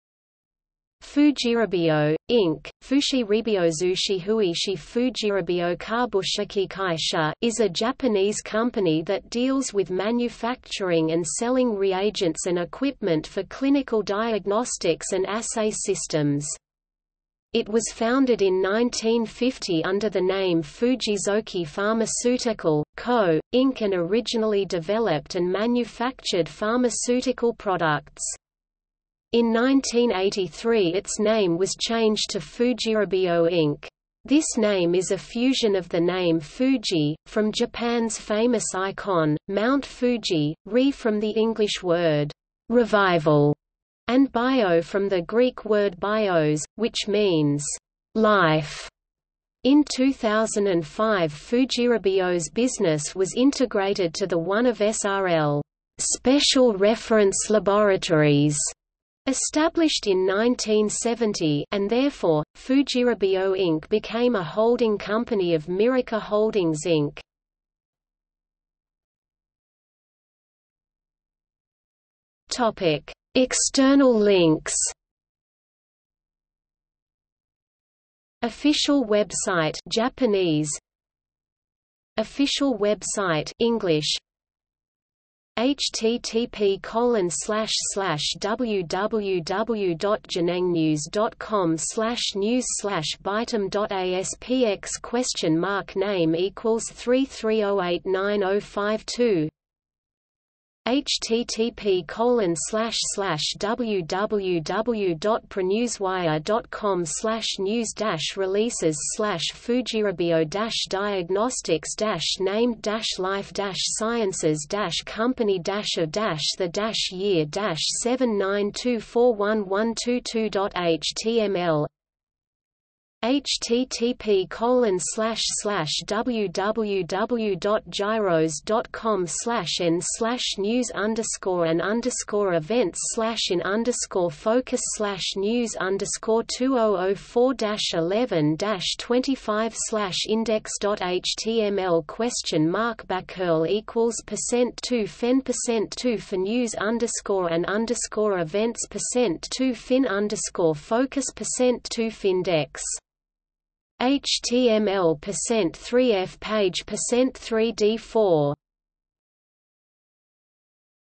Fujirebio | Wikipedia audio article
00:01:09 External links

- Socrates

SUMMARY
Fujirebio, Inc. (富士レビオ株式会社, Fujirebio Kabushikikaisha) is a Japanese company that deals with manufacturing and selling reagents and equipment for clinical diagnostics and assay systems. It was founded in 1950 under the name Fujizoki Pharmaceutical, Co., Inc. and originally developed and manufactured pharmaceutical products. In 1983 its name was changed to Fujirebio Inc. This name is a fusion of the name FUJI, from Japan's famous icon, Mount Fuji, RE from the English word "revival", and BIO from the Greek word bios, which means "life". In 2005 Fujirebio's business was integrated to the one of SRL ("Special Reference Laboratories", established in 1970) and therefore, Fujirebio Inc. became a holding company of Miraca Holdings Inc.